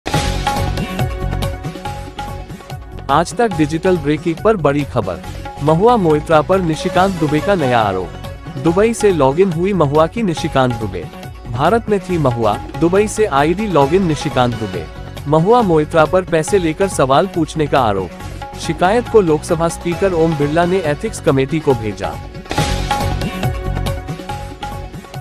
Breaking News: 'Mahua जब भारत में थीं, तो उनकी लोकसभा ID दुबई से हुई लॉगिन', Nishikant Dubey का आरोप
तृणमूल कांग्रेस की सांसद महुआ मोइत्रा इन दिनों सुर्खियों में हैं. महुआ पर एक के बाद एक आरोप लग रहे हैं जिससे उनकी मुश्किलें बढ़ती जा रही हैं. बीजेपी सांसद निशिकांत दुबे ने एक बार फिर महुआ पर गंभीर आरोप लगाया है. उन्होंने कहा कि जब महुआ भारत में थी तो उनकी लोकसभा आईडी दुबई से लॉगिन हुई थी.